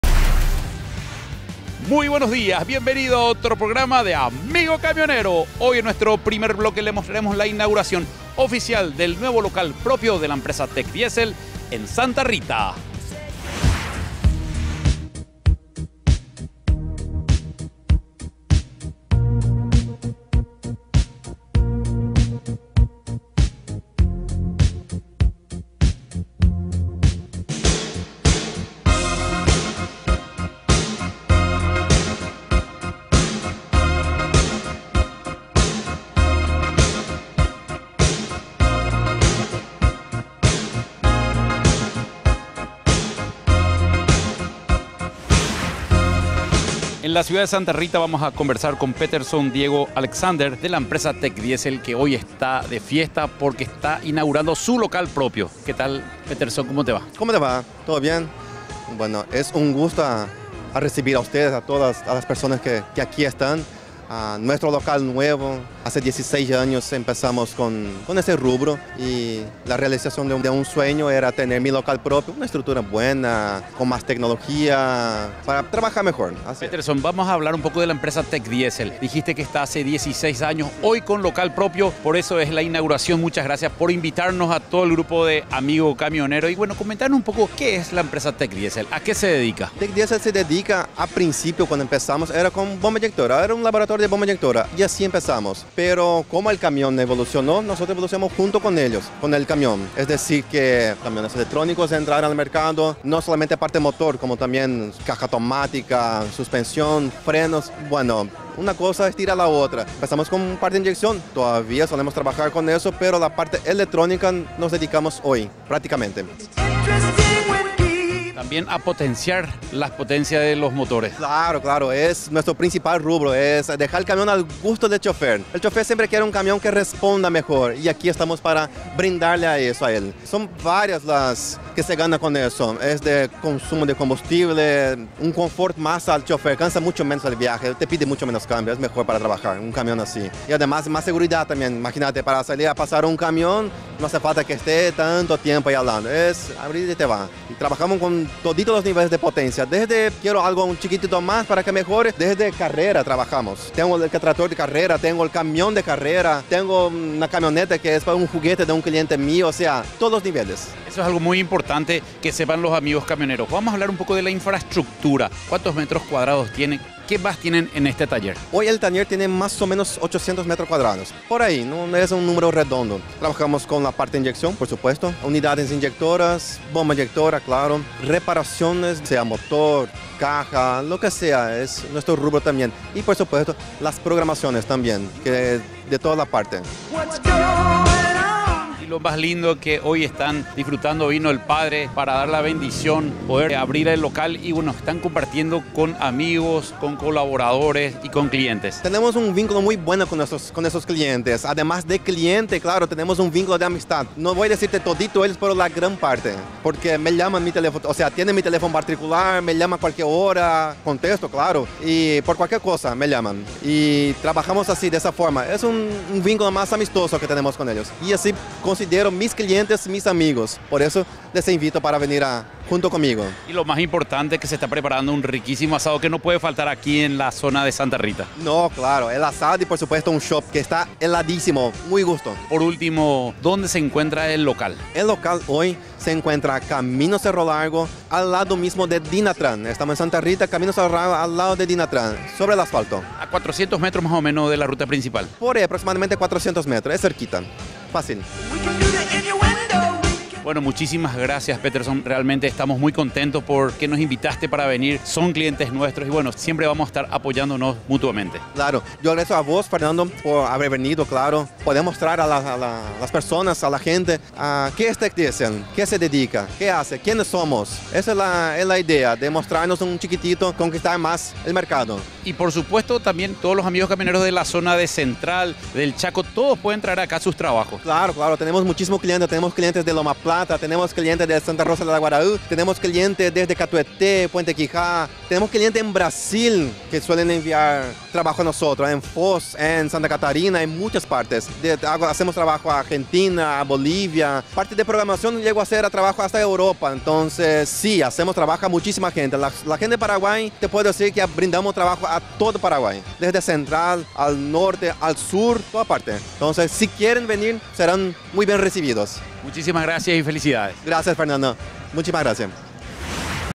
TECDIESEL inauguró nuevo local propio en Santa Rita
En una amena celebración con amigos, clientes y colaboradores, TECDIESEL inauguró su nuevo local ubicado en la ciudad de Santa Rita, camino a Cerro Largo a 400mts de la Ruta PY06.

La construcción del taller del nuevo local tiene 812 metros cuadrados, para brindar todas las comodidades a los clientes, con instalaciones de primer nivel y un equipo técnico altamente capacitado para brindar servicios de reparación y mantenimiento de bombas inyectoras, inyección electrónica y diagnóstico electrónico para camiones en general.

Además, el nuevo local de Santa Rita ya cuenta con los servicios de reprogramación de potencia, reparación de módulo de Inyección (Caja Negra), Sistemas de Inyección EDC- PDE- HPI- XPI, entre otros.

TECDIESEL es una empresa que está presente en el mercado paraguayo desde hace 16 años, brindando el servicio de inyección electrónica para camiones.